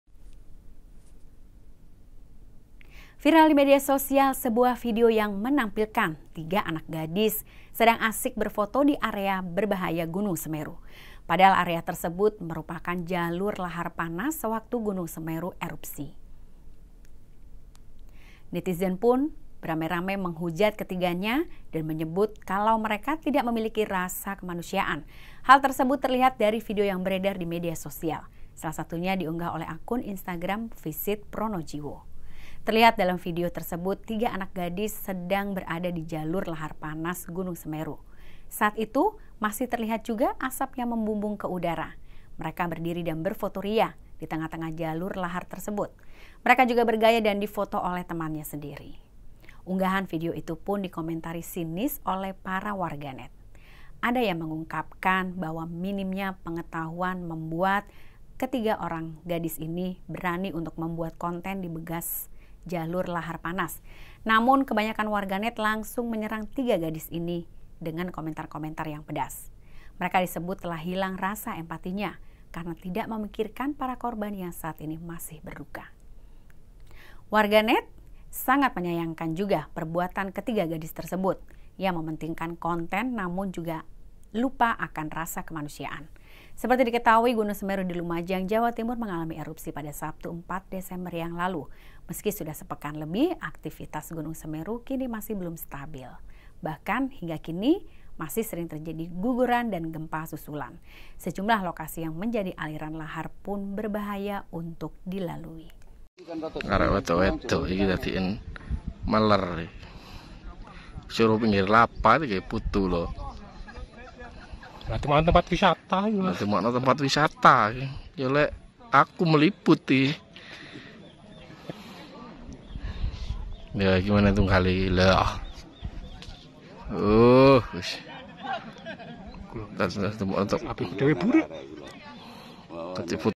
Viral Sejumlah Muda-Mudi Asyik Berfoto di Area Berbahaya Bekas Aliran Lahar Gunung Semeru
Padahal area tersebut merupakan jalur lahar panas sewaktu Gunung Semeru erupsi.

Nitizen pun beramai-ramai menghujat ketiganya, dan menyebut mereka tidak memiliki rasa kemanusiaan.